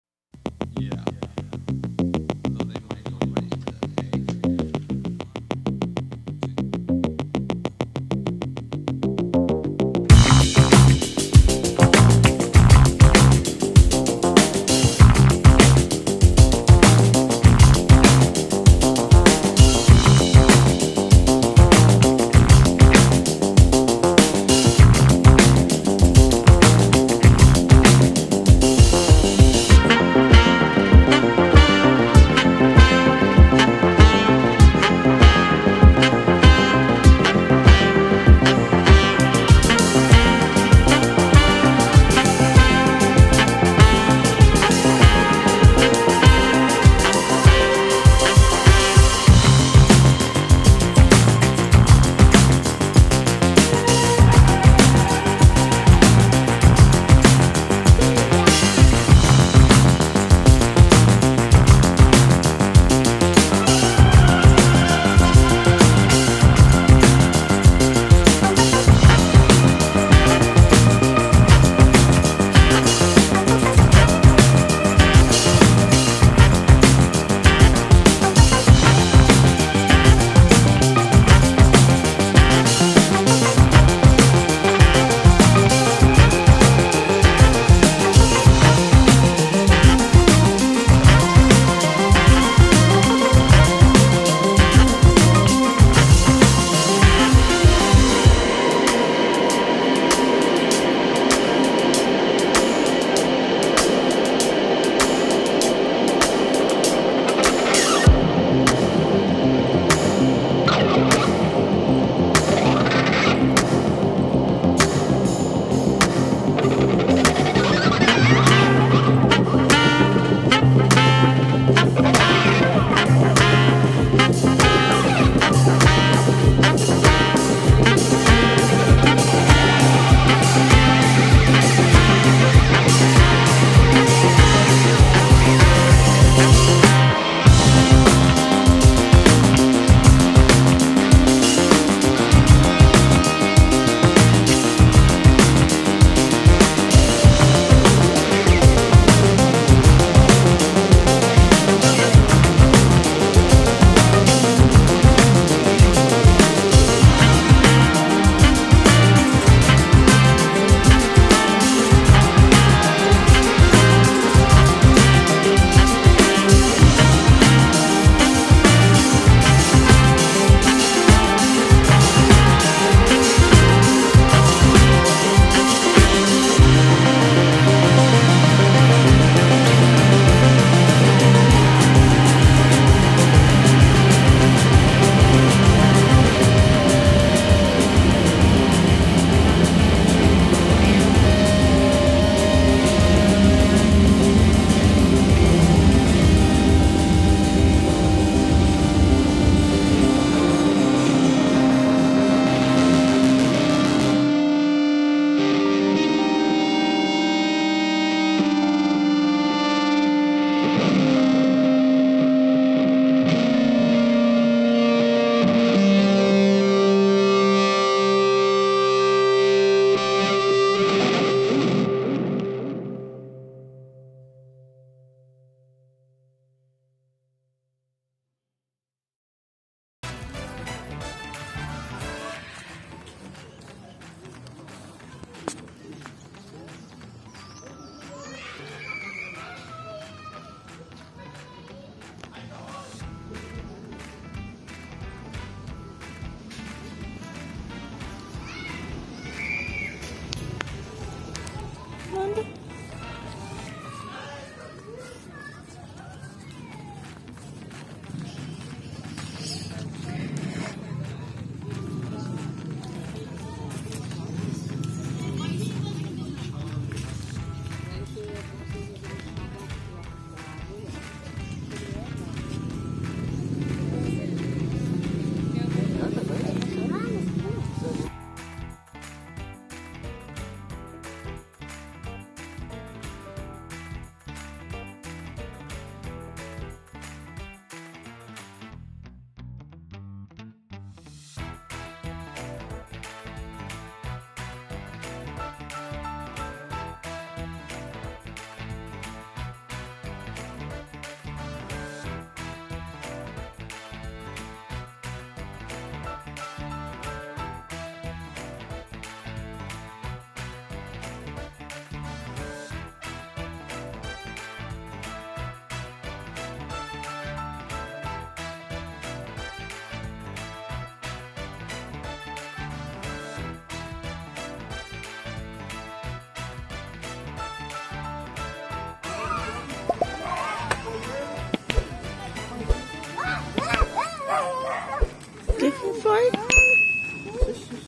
ESCALEI A NAVE DE BAMBU NO PARQUE !
O brinquedo de bambu para descobrir a escalada foi muito divertido!
Ao escalar a árvore, subir num tronco, Miguelito descobriu seus medos e desenvolveu a auto-confiança com o incentivo e apoio do papai! Venha conhecer este divertido passeio com escalada!